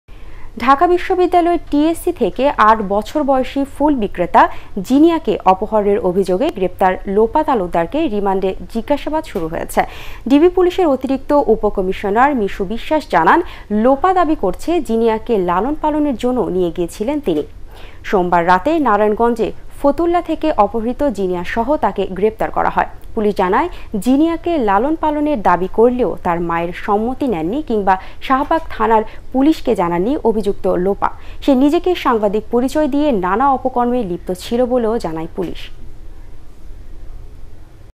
ফুল বিক্রেতা জিনিয়াকে অপহরণের অভিযোগে গ্রেপ্তার লোপা তালুকদারকে রিমান্ডে জিজ্ঞাসাবাদ শুরু
ফুল বিক্রেতা জিনিয়াকে অপহরণের অভিযোগে গ্রেপ্তার লোপা তালুকদারকে রিমান্ডে জিজ্ঞাসাবাদ শুরু | ATN Bangla